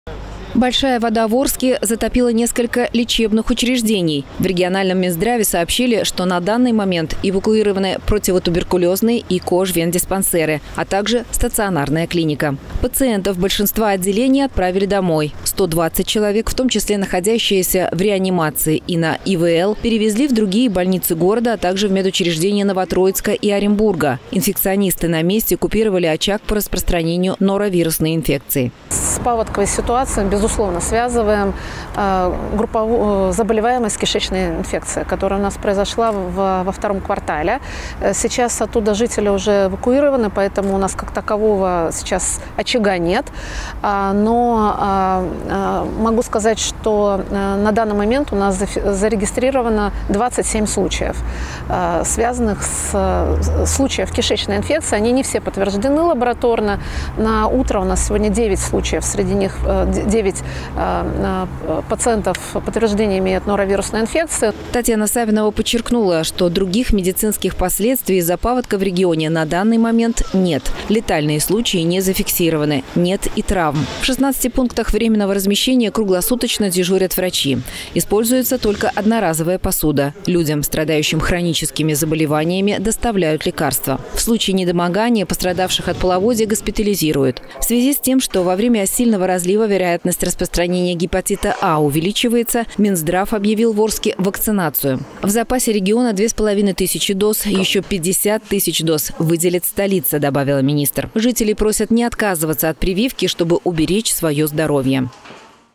ВИЦЕ-ГУБЕРНАТОР ТАТЬЯНА САВИНОВА РАССКАЗАЛА О ПОСЛЕДСТВИЯХ ПАВОДКА В ОРСКЕ ДЛЯ ЗДОРОВЬЯ
Смертей и травм нет, но случаи заражения кишечной инфекцией есть. О последствиях затопления Орска рассказала вице-губернатор и министр здравоохранения Оренбургской области Татьяна Савинова. Она также пояснила, как медики намерены помогать жителям, которые имеют хронические заболевания, и тем, кто потерял свои дома.